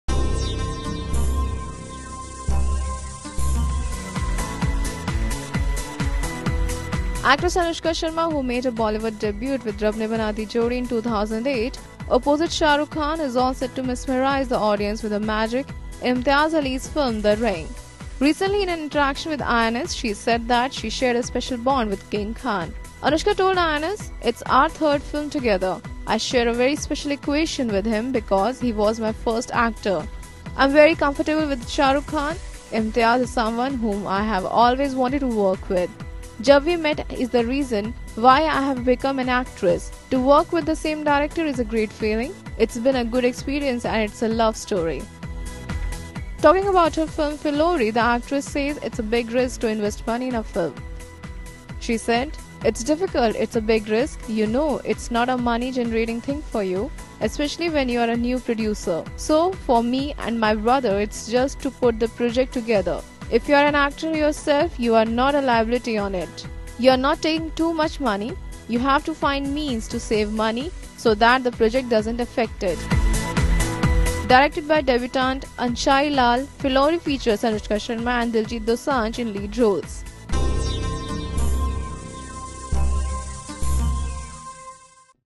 Anushka Sharmaís REVELATION about SRK
Actress Anushka Sharma who made her Bollywood debut with ëRab Ne Bana Di Jodií in 2008 opposite Shah Rukh Khan, is all set to mesmerize the audience with her magic in Imtiaz Aliís film "The Ring". Recently, in an interaction with IANS, she said that she shared a special bond with King Khan.